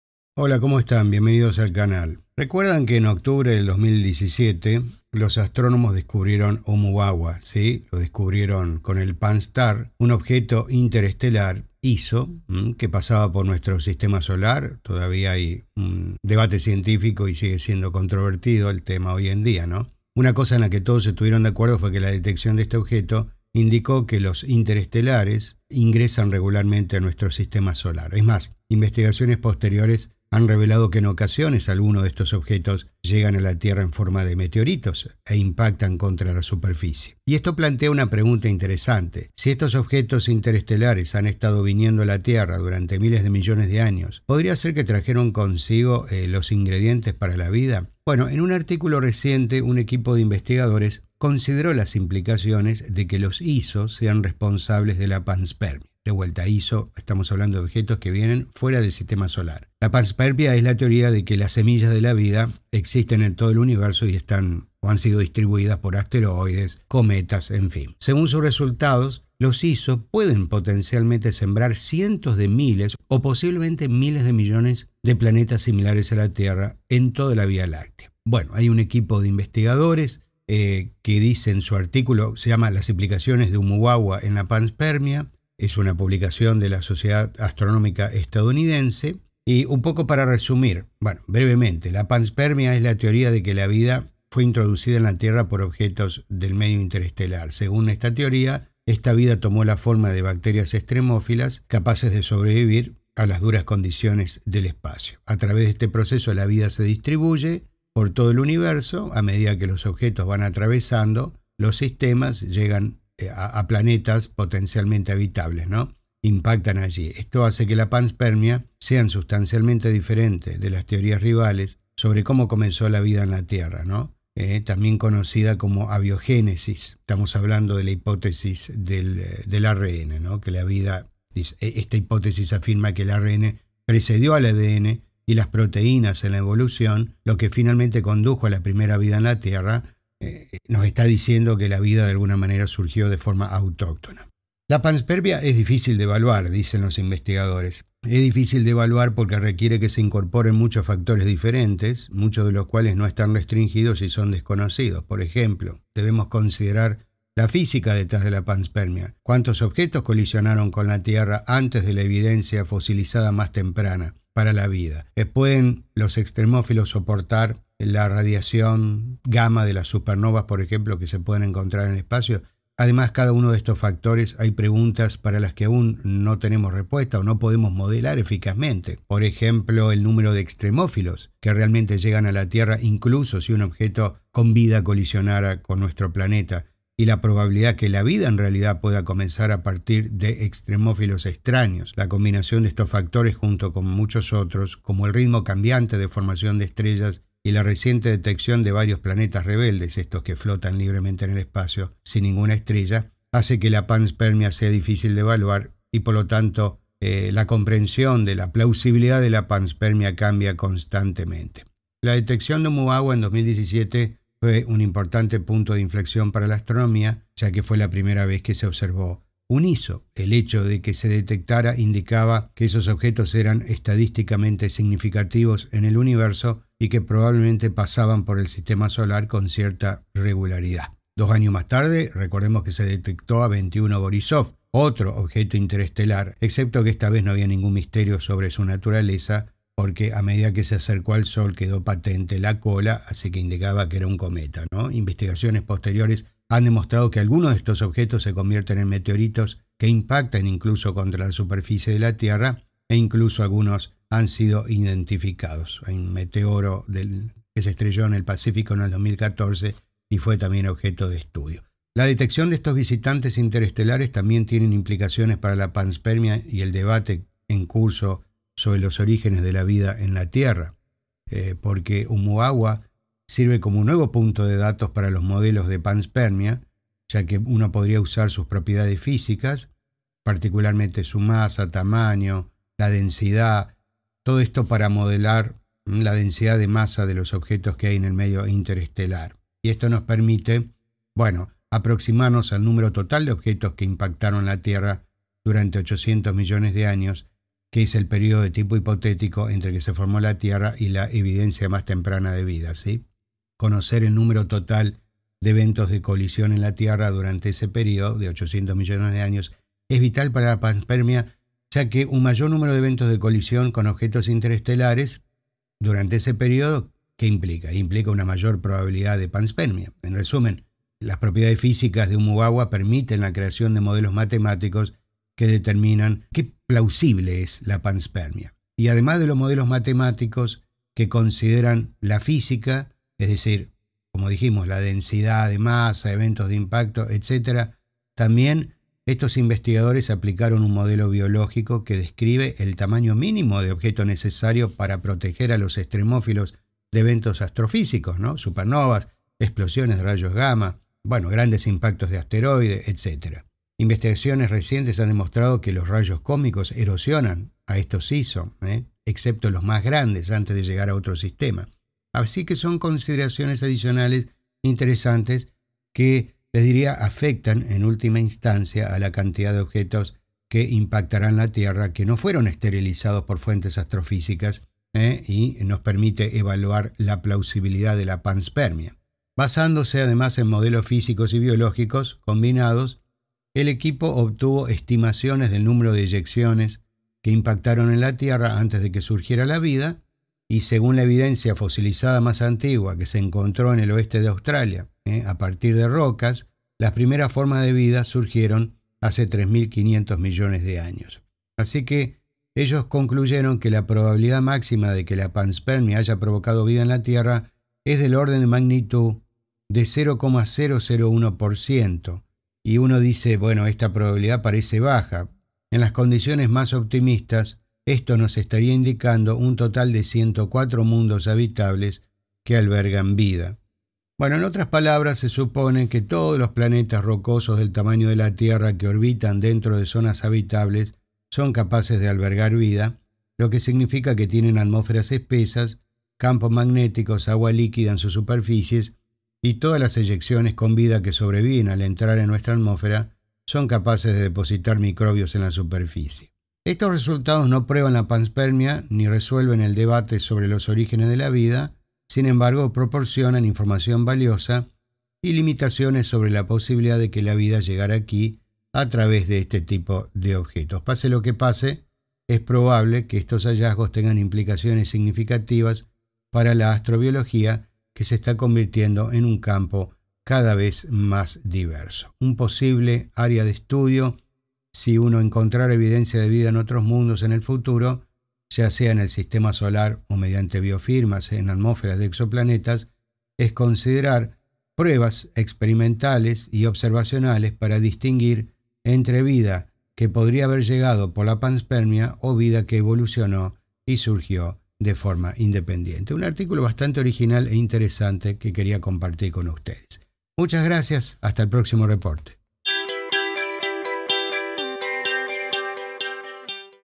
LA VIDA EN LA TIERRA COMENZÓ CUANDO OBJETOS INTERESTELARES COMO OUMUAMUA SE ESTRELLARON AQUI?
El 19 de octubre de 2017 , los astrónomos del estudio Pan-STARRS detectaron por primera vez un objeto interestelar (ISO) que pasaba por nuestro Sistema Solar. El objeto, conocido como 1I/2017 U1 Oumuamua, suscitó un importante debate científico y sigue siendo controvertido hoy en día.